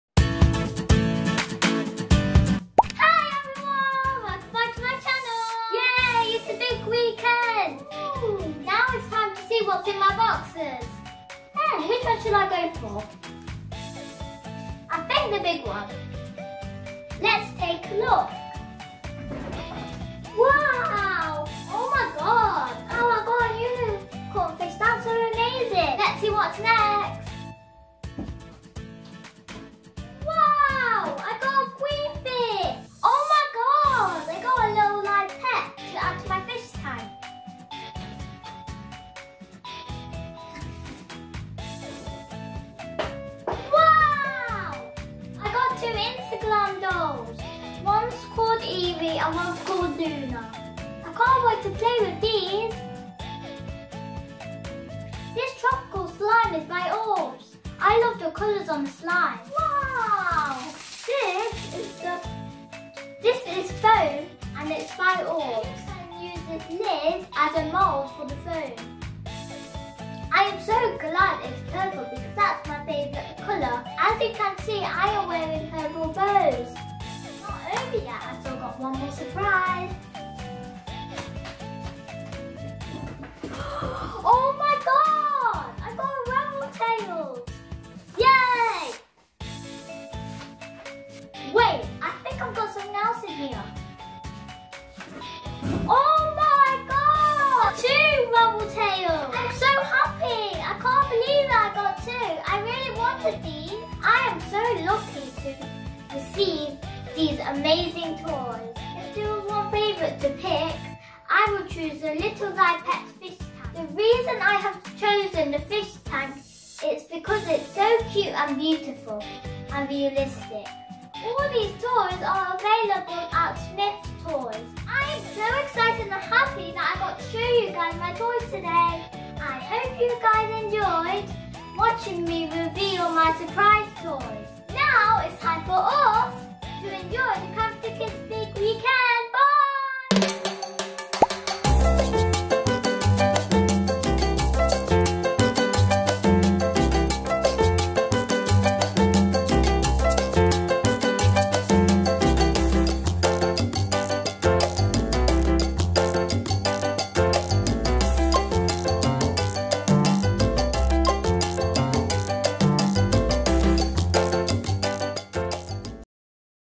The Character Kidz Big Weekend What's In My Surprise Box Revealed... 👀🤩🌈
It's Time To Find Out Guys...👀🤩

The Character Kidz Big Weekend is a virtual toy launch event and guess what guys... I was invited along, Yayyy!!! 🎉🎉🎉

I got the chance to check out the latest toys.

The Character Kidz sent me a huge surprise box full of the newest toys in town to check out and review.

All toys featured in this video are available from Smyth Toys Stores 😀

Come take a look to see what's in my box 🤩